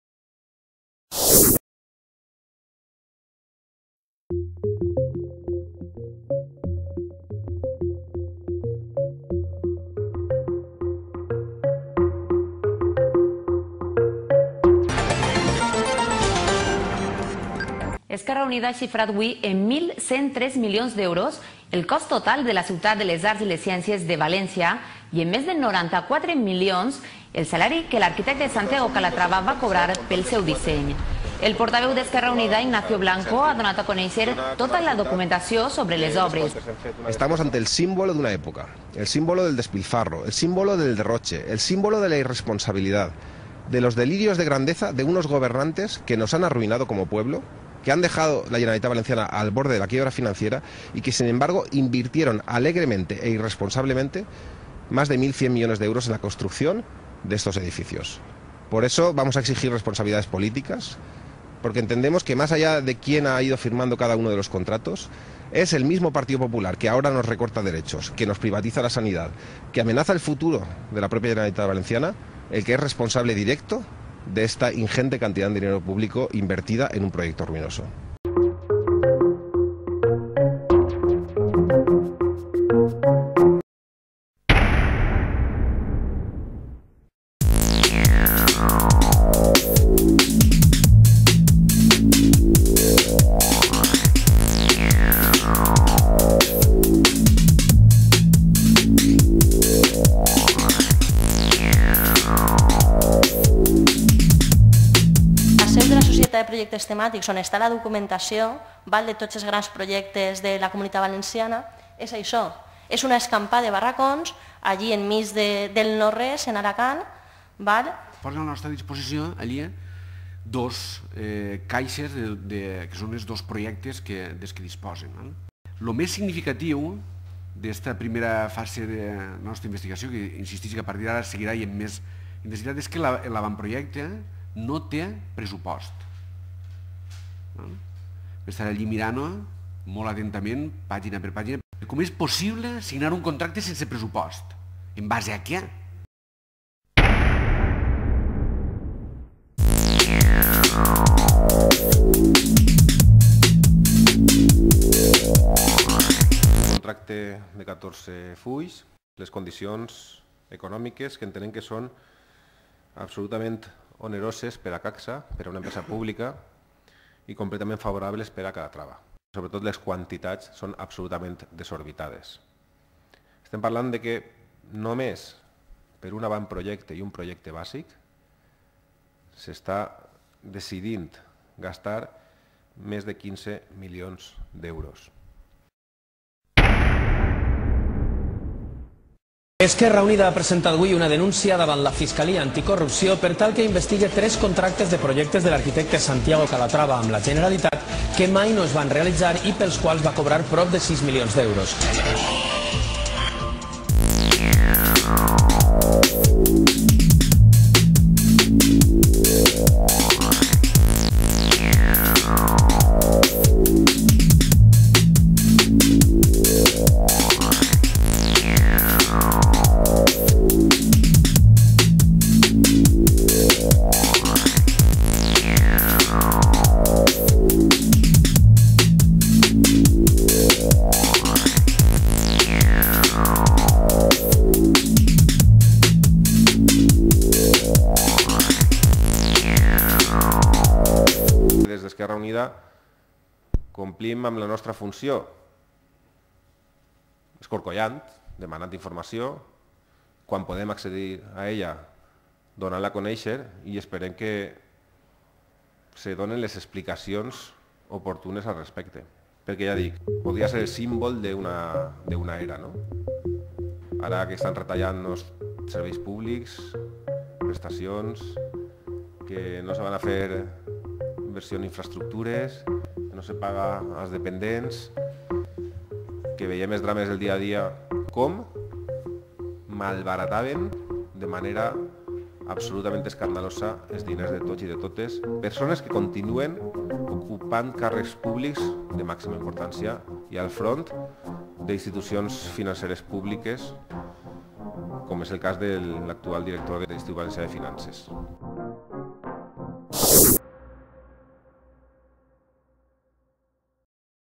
CALATRAVATELACLAVA.COM: projectes ruïnosos sense IVA amb diners públics
Esquerra Unida fa retrospectiva del seu treball d'investigació durant els vora dos anys que han sigut necessaris per aconseguir accedir als principals contractes de la Generalitat amb l'arquitecte Santiago Calatrava. El resultat? Un sorprenent balafiament de diners públics en projectes ruïnosos, amb forts sobrecostos sobre el preu original de l'obra que, per a fer 'pàtria', van ser pagats sense IVA a Calatrava... Servisca d'exemple la Ciutat de les Arts i les Ciències (CACSA), per la qual el Govern valencià va pagar uns 1.100 milions d'euros dels quals l'arquitecte s'embutxacà 94 milions.